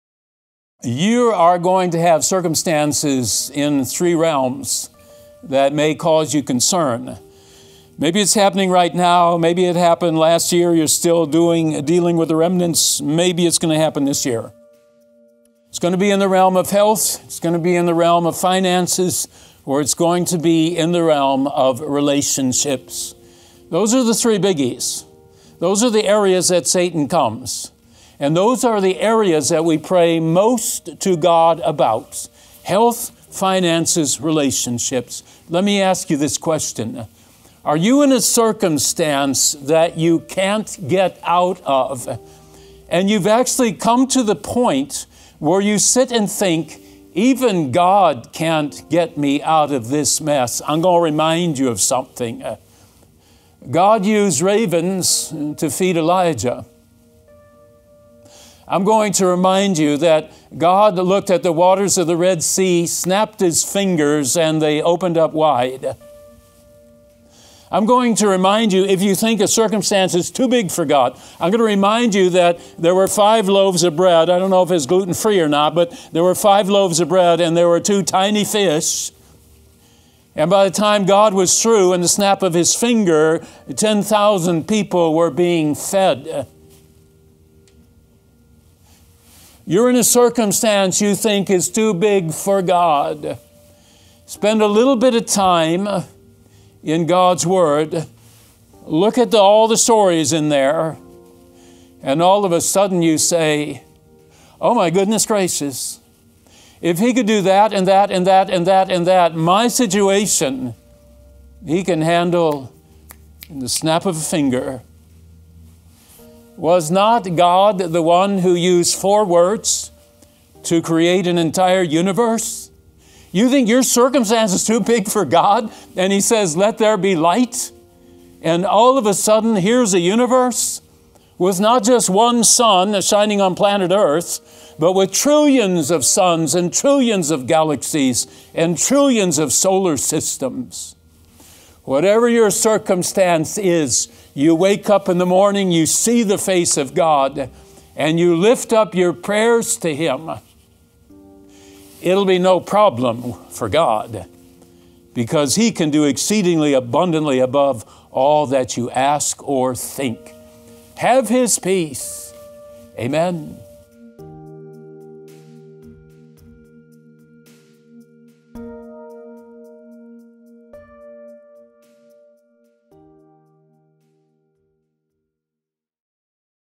Have His Peace | Pastor Paul Strand VLOG 116
"Have His Peace"
Pastor Paul Strand
VLOG 116
Ephesians 3:20
April 1st  2021
Trinity Lutheran Church
Tinley Park, IL